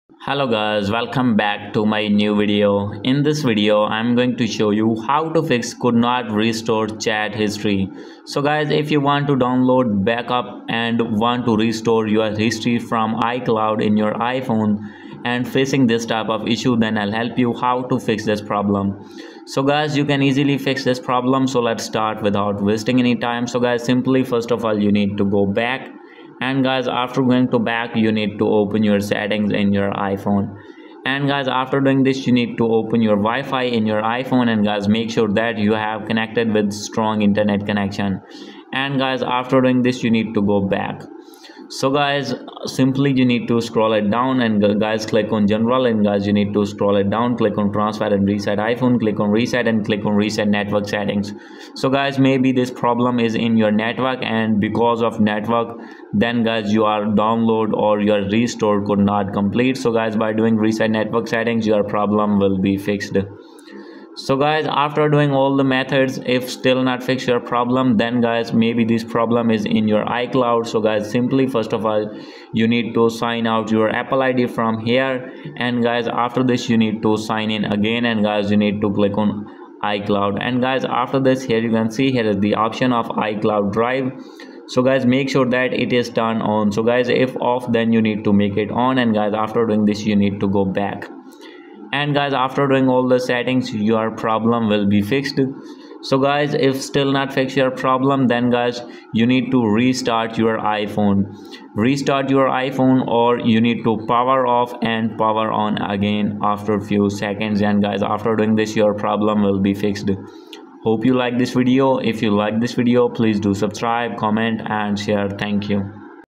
How To Fix Couldn't Restore Chat History On WhatsApp iPhone
In this video, I will help you to fix couldn't restore chat history whatsapp iphone. Please watch till the end.